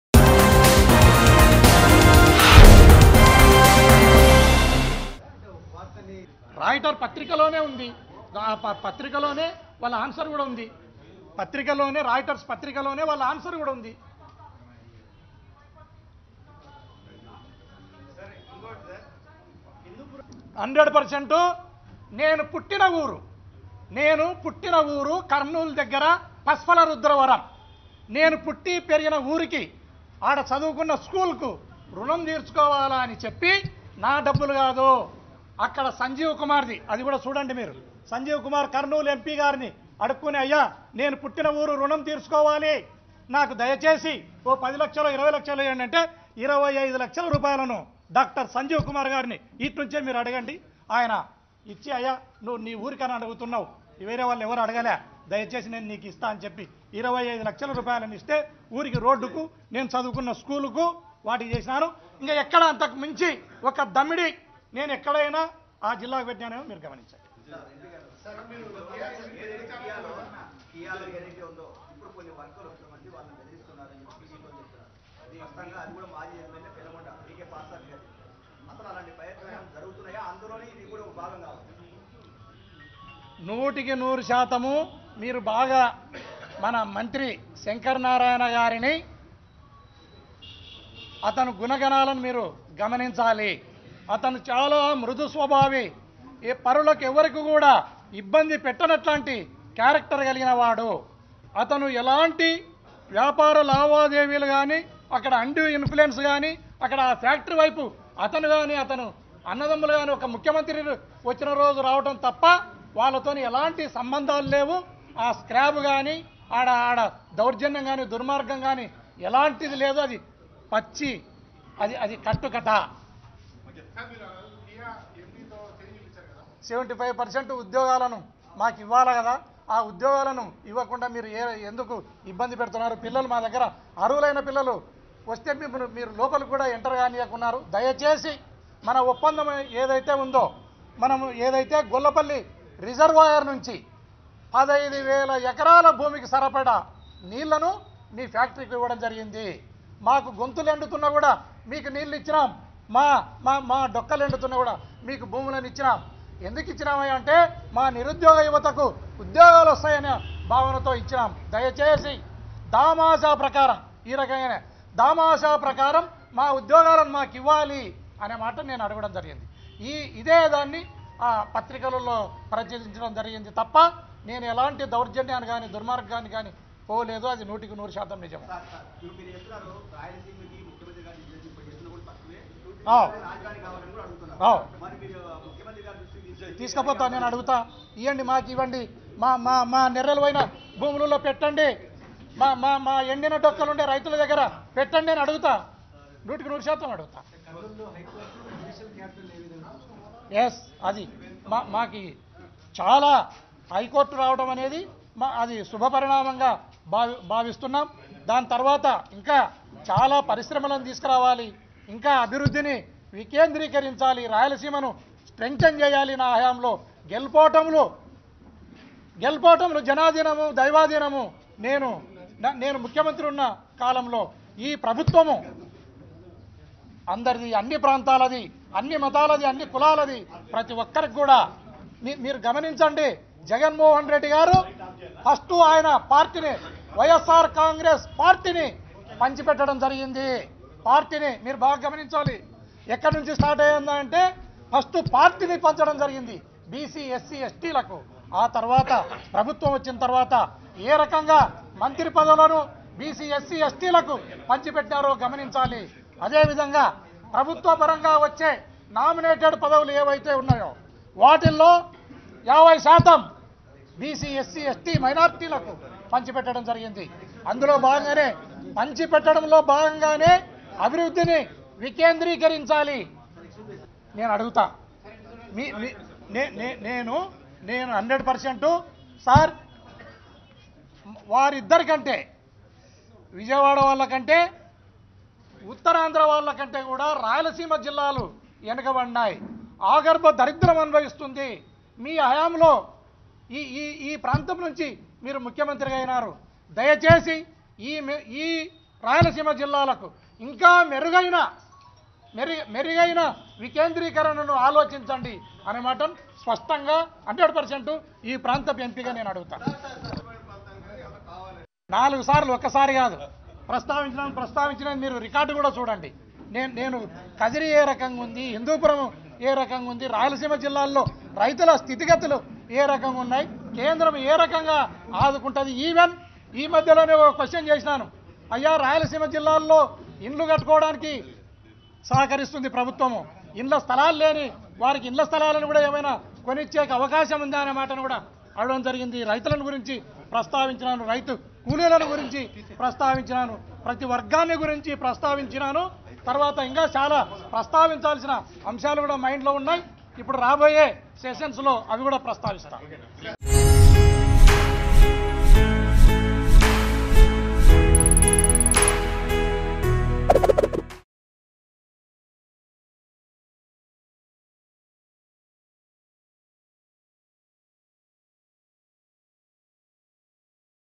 Hindupur MP Gorantla Madhav Gives Clarification Over KIA Motors Issue In Press Meet | Mango News
Hindupur MP Gorantla Madhav Gives Clarification Over KIA Motors Issue In Press Meet.